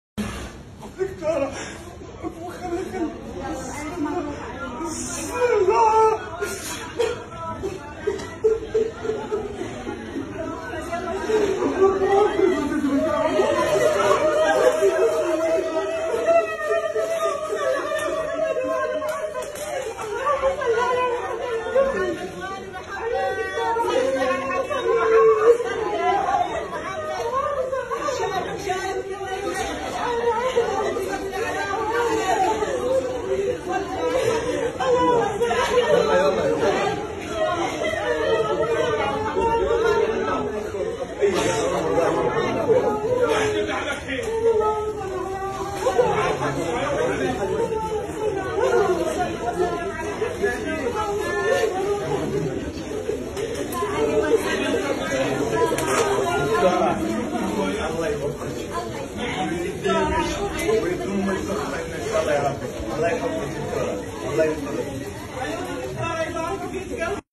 ایک بھائی کو اللہ تعالٰی نے اٹھارہ 18سال بعد اولاد عطا کیا بھائی اور اس کی خاندان کی خوشی کے مناظر.
ایک بھائی کو اللہ تعالٰی نے  اٹھارہ (18)سال بعد اولاد عطا کیا
بھائی اور اسکی خاندان کی خوشی کے مناظر دیکھیں